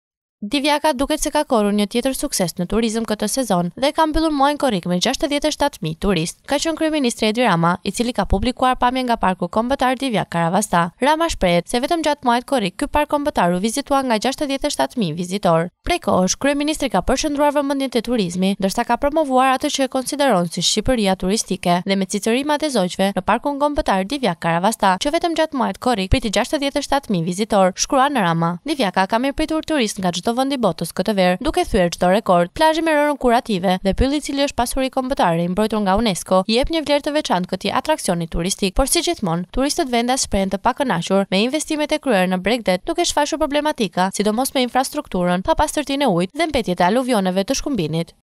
DIVJAKA MBYLL ME SUKSES MUAJIN KORRIK ME MBI 67 MIJË VIZITORË NGA E GJITHË BOTA
Lajmet në RTV 4Plus Lushnjë
Gazetarë:
S.Zgjana
J.Kasapi
M.Topi

Operator:
G.Laboti

Teknik/Montazh:
G.Koçiu